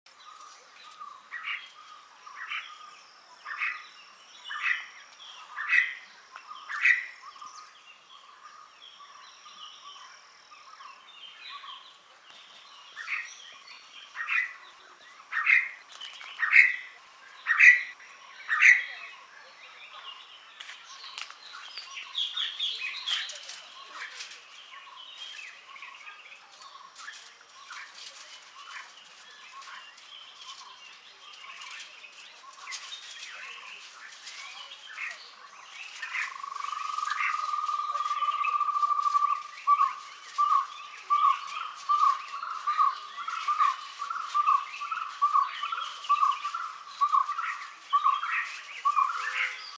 Red spurfowl (Galloperdix spadicea) calling - Typical call of red spurfowl
Red spurfowl (Galloperdix spadicea) calling - Typical call of red spurfowl. The calls include a distinct ker-wick...kerwick... and harsh karr...karrr... notes. The Marathi name Kokatri is echoic in origin.
Scientific classification:
Kingdom:   Animalia
Phylum:      Chordata
Class:         Aves
Order:         Galliformes
Family:       Phasianidae
Genus:       Galloperdix
Species:    G. spadicea